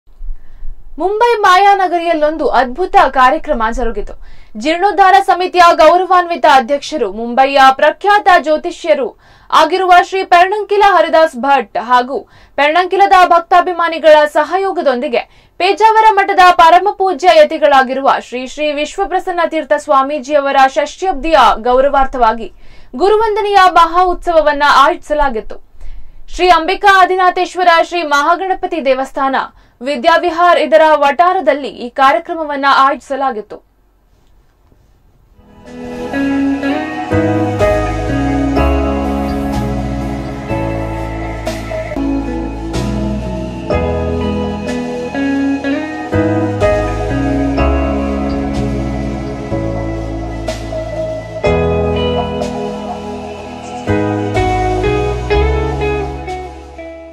ಮುಂಬೈ - ಗುರುವಂದನೆಯ ಮಹಾ ಉತ್ಸವ
ಮುಂಬೈ ಮಾಯಾ ನಗರಿಯಲ್ಲೊಂದು ಅದ್ಭುತ ಕಾರ್ಯಕ್ರಮ ಜರುಗಿತು. ಜೀರ್ಣೋದ್ಧಾರ ಸಮಿತಿಯ ಗೌರವಾನ್ವಿತ ಅಧ್ಯಕ್ಷರು ಮುಂಬೈಯ ಪ್ರಖ್ಯಾತ ಜ್ಯೋತಿಷ್ಯರೂ ಆಗಿರುವ ಶ್ರೀ ಪೆರ್ಣಂಕಿಲ ಹರಿದಾಸ ಭಟ್ ಹಾಗೂ ಪೆರ್ಣಂಕಿಲದ ಭಕ್ತಾಭಿಮಾನಿಗಳ ಸಹಯೋಗದೊಂದಿಗೆ ಪೇಜಾವರ ಮಠದ ಪರಮ ಪೂಜ್ಯ ಯತಿಗಳಾಗಿರುವ ಶ್ರೀ ಶ್ರೀ ವಿಶ್ವ ಪ್ರಸನ್ನ ತೀರ್ಥ ಸ್ವಾಮೀಜಿಯವರ ಷಷ್ಟ÷್ಯಬ್ದಿಯ ಗೌರವಾರ್ಥವಾಗಿ ಗುರುವಂದನೆಯ ಮಹಾ ಉತ್ಸವವನ್ನು ಆಯೋಜಿಸಲಾಗಿತ್ತು.